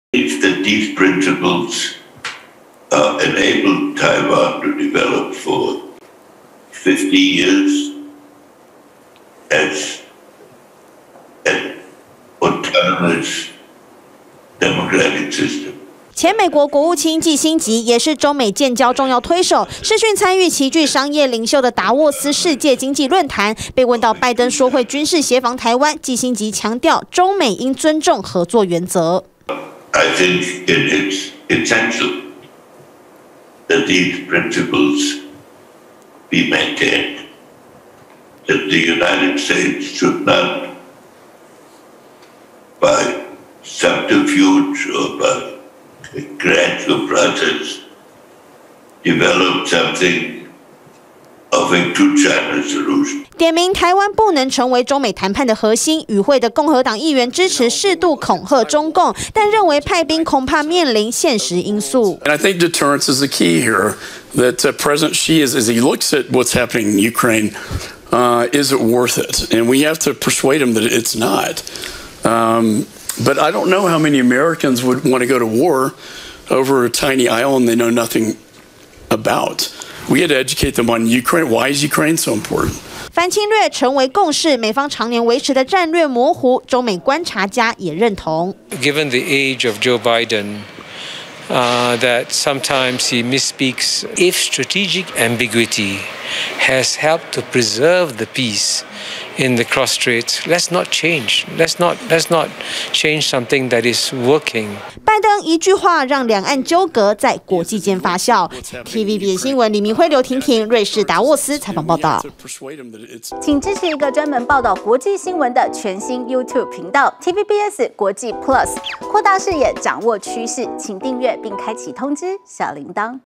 獨家 回應拜登! 季辛吉 台不能成美中談判核心｜TVBS新聞
在美國總統拜登說會軍事協防台灣後，同一時間在瑞士達沃斯展開的經濟論壇也紛紛討論；其中，前美國國務季辛吉認為，美中談判應避免將台灣置於核心。而TVBS在瑞士獨家採訪中美觀察家，則認為美方不該改變能維持兩岸和平的「戰略模糊」。